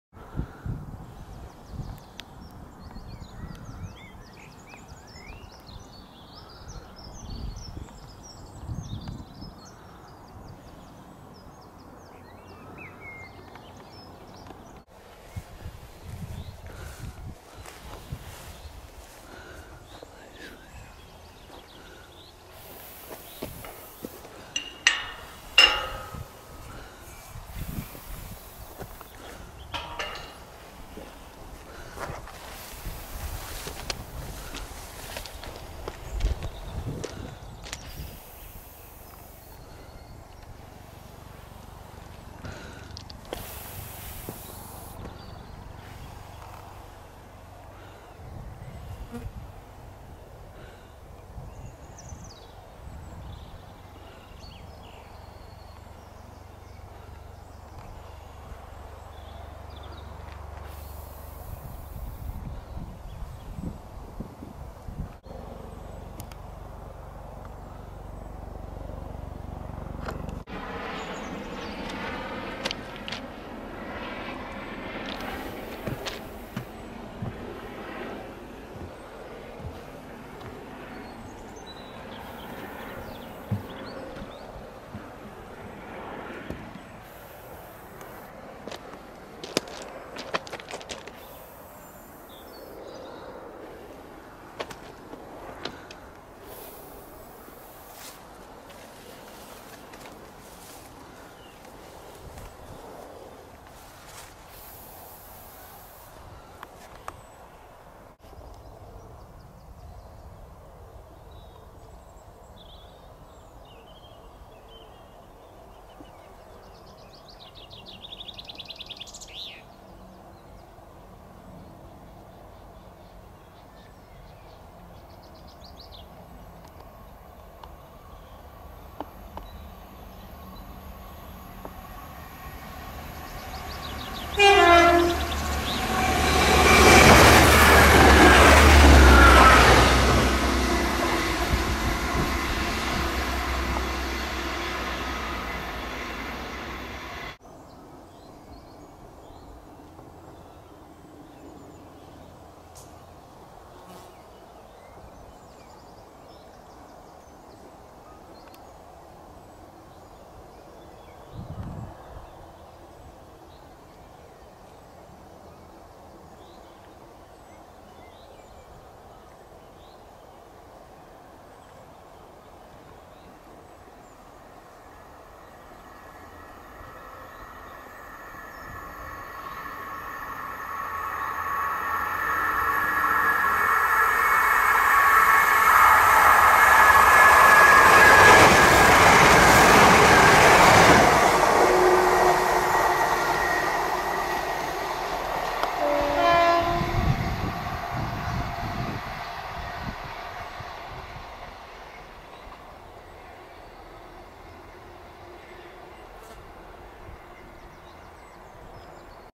Dowdes Foot Crossing
Thursday June 10 2010 at 12.20.  The 11.20 Reading to Gatwick Airport (13.16) and the 12.29 Gatwick Airport to Reading (13.51) trains.   Nearly half a mile of the 2nd approaching train is shown, hence the heat haze, and no horn sounded.  Maybe the sign has gone.  There is no sign in the other direction.  This is 600 yards from Church foot crossing and at 1.09 on a large screen you can see a lorry on Reigate Level crossing 1.5 miles away.   The crossing has been repaired recently with a new kissing gate installed just days earlier.  Only one person a day uses this crossing apparently.  Nevertheless Network Rail’s ludicrous risk assessment system rates it as ‘Very High Risk’.  In a magical scene which could have come from 'The Railway Children' film, 50 years ago a train driver waved at my friend and I as we waited at crossing somewhere nearby.